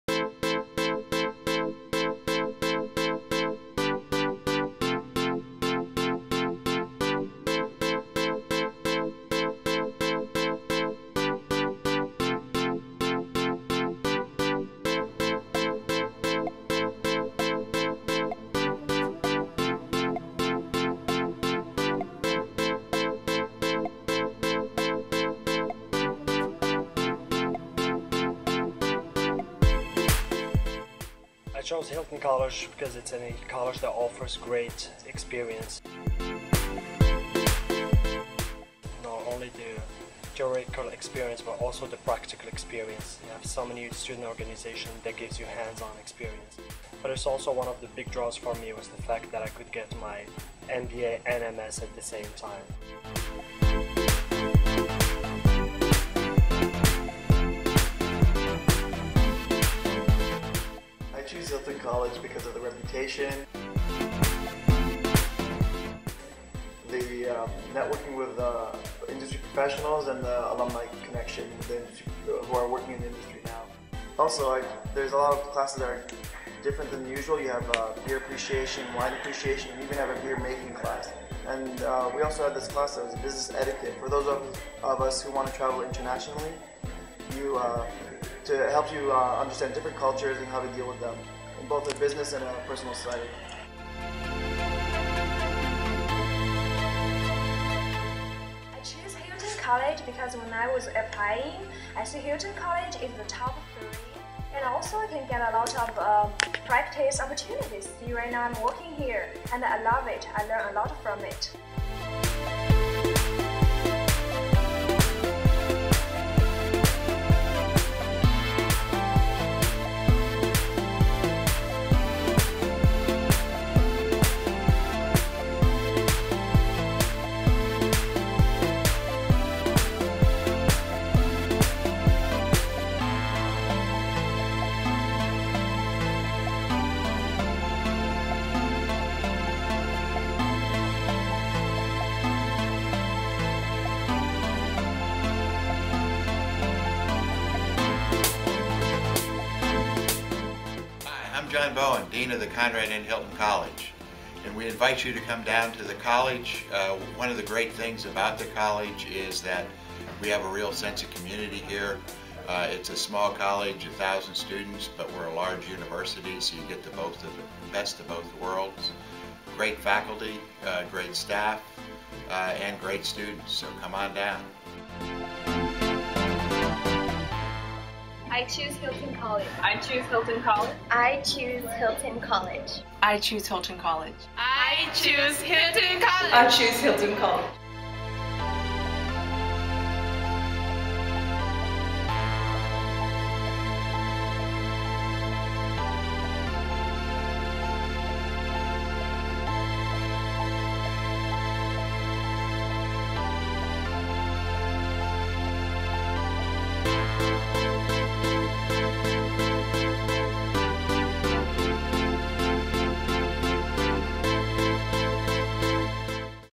Hilton College Promo.mov
This is about the Conrad N. Hilton College.  It shows many activities, students, and the facility.